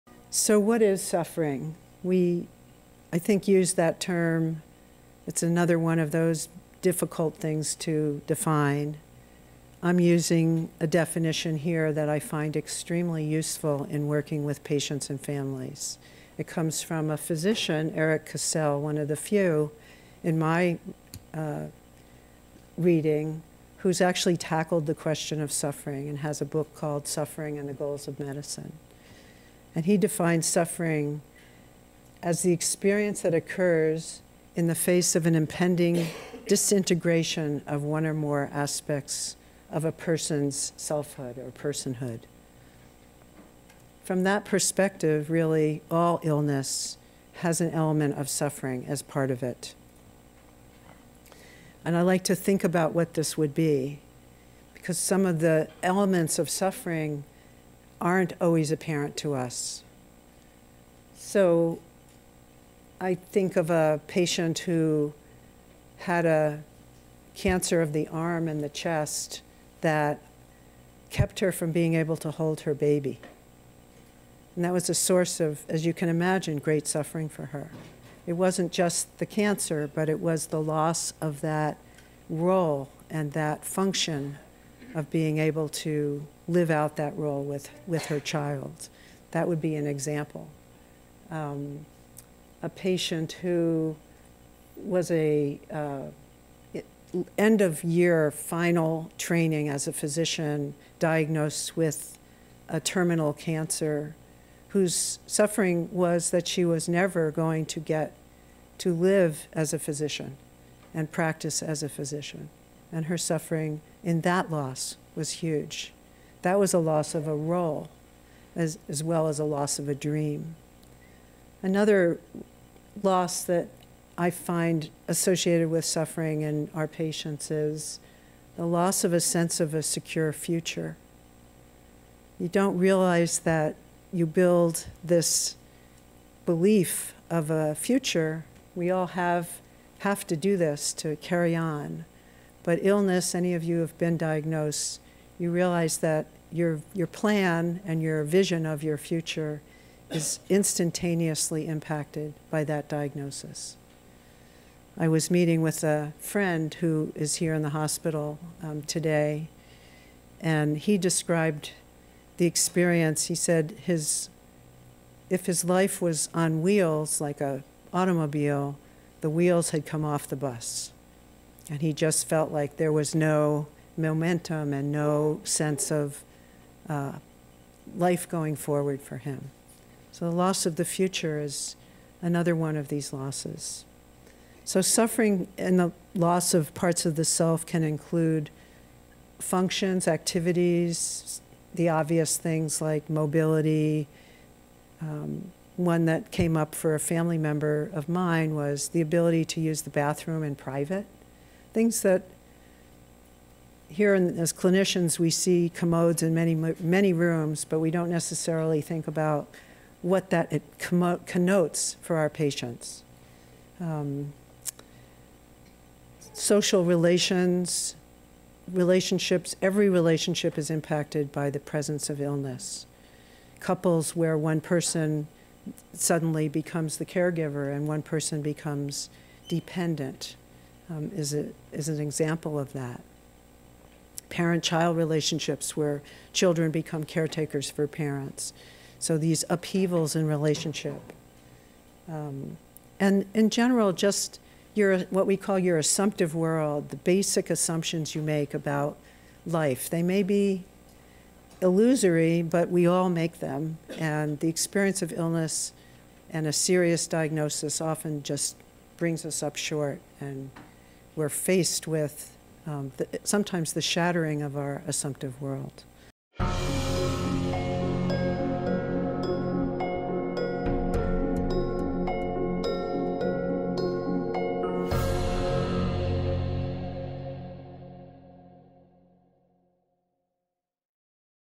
Illness, Identity and Suffering
Rev. Denah Joseph, Chaplain and Associate Director UCSF Palliative Care Service, discusses the psychology of suffering when serious illness or loss occurs. [7/2018] [Show ID: 33866]